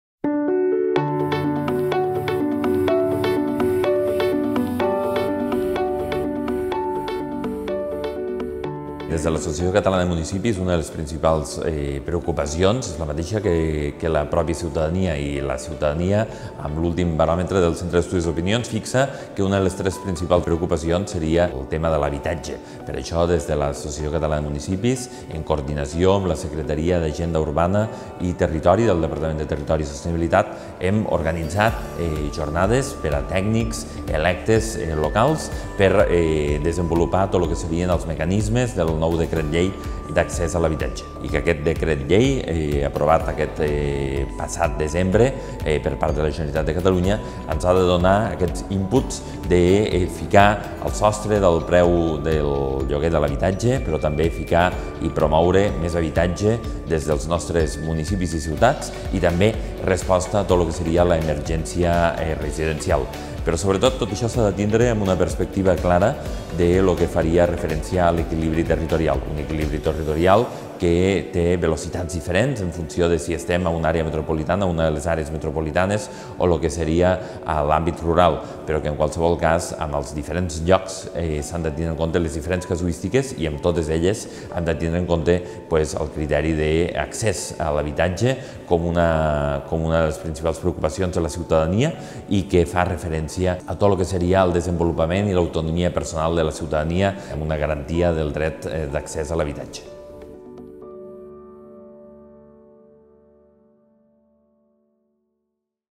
L'habitatge és un dret social i una eina d'equilibri territorial.
🏠 L'habitatge és un dret social i de desenvolupament humà de màxima importància:
✅ Des del món local hem de fomentar l'accés a l'habitatge assequible, digne, accessible i sostenible,
🔴 Atendre a les situacions d'emergència habitacional, tant pel que fa als desnonaments com a les situacions de pobresa, amb el "housing first".
📍 Fomentar l'equilibri territorial, incoporant l'Agenda Rural i la lluita contra el despoblament.